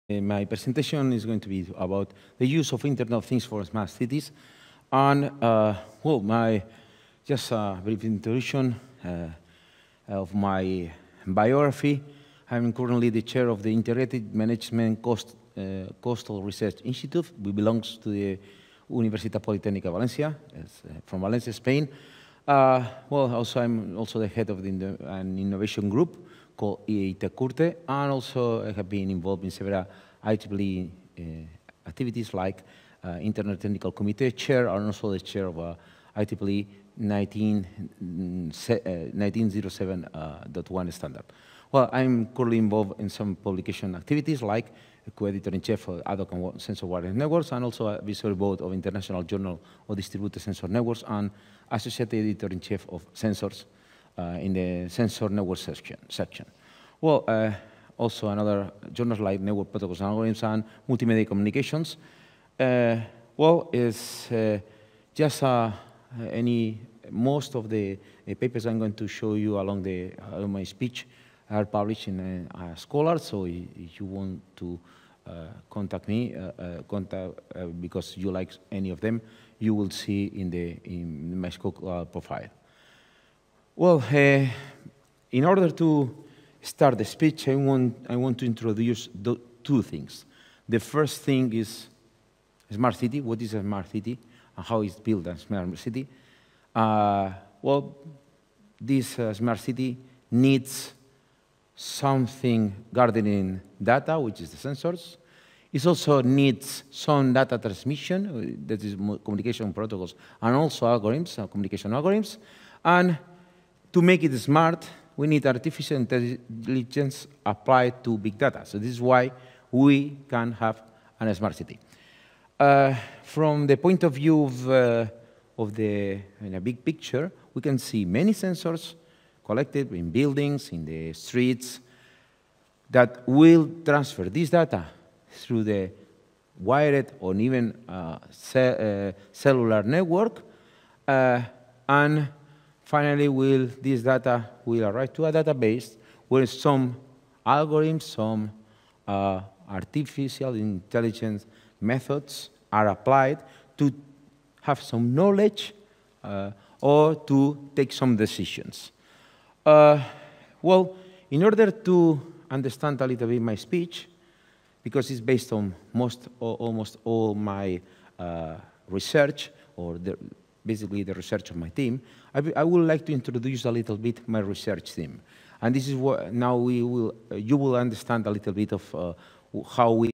"The Use of Internet of Things for Smart..." Prof. Jaime Lloret Mauri (PECCS 2018)
Title: The Use of Internet of Things for Smart Cities

Keynote Lecturer: Jaime Lloret Maur
Presented on: 30/07/2018, Porto, Portugal

Abstract: Recent advances in new technologies jointly with the appearance of low cost sensors with computing and communication capacities have made possible to implement new systems to improve citizens daylife. Internet of Things allows monotiring from everywhere at any time. This speech will review recent developments of sensors and internet of things to gather data from urban environments and which network protocols, algorithms and architectures have been designed and developed to provide the most updated data. The knowledge acquired by these sensors can be tackled in order to improve the electric consumption, the water wastage and even any type of lakes in gas, electricity or water. Big data and artificial intelligence techniques can be used to optimize the resources of the cities and improve their performance. The talk will explain several examples and provide real implementations.

Presented at the following Event:
PECCS, 8th International Joint Conference on Pervasive and Embedded Computing and Communication Systems